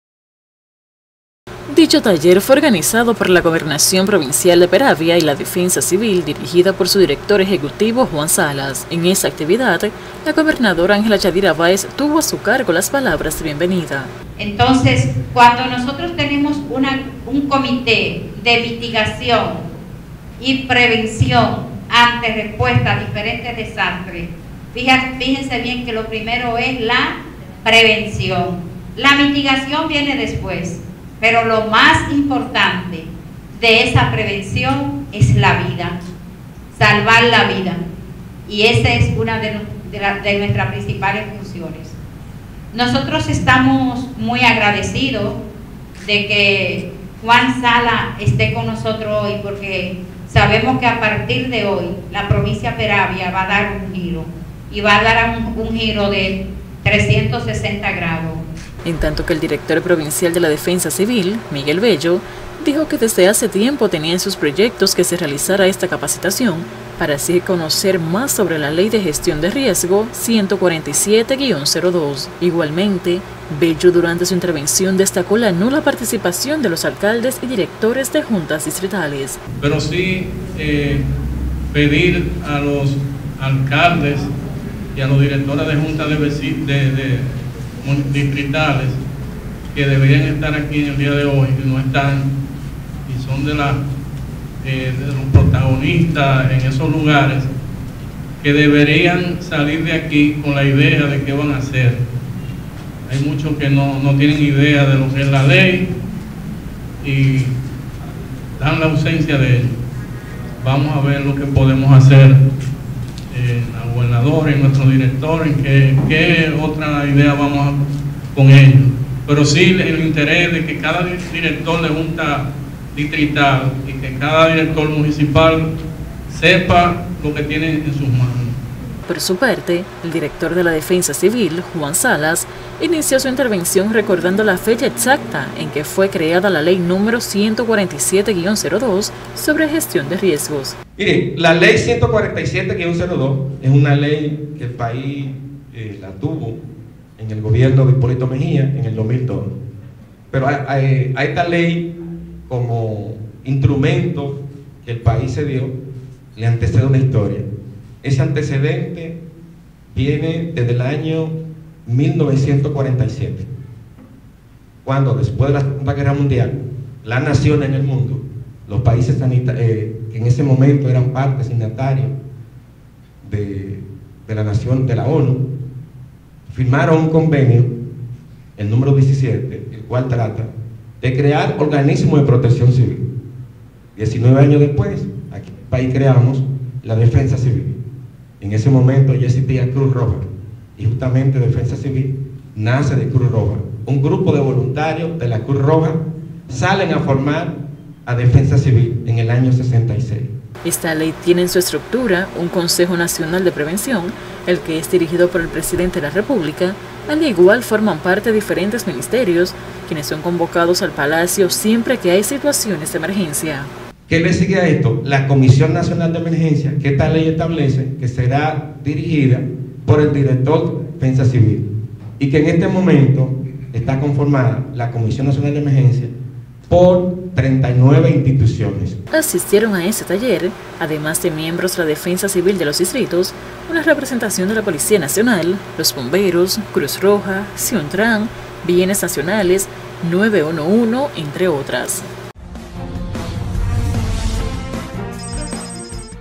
Realizan taller sobre ley 147-02 de Gestión de Riesgo
Este jueves el salón de actos de la gobernación provincial fue escenario para la realización del taller sobre la Ley de Gestión de Riesgo 147-02.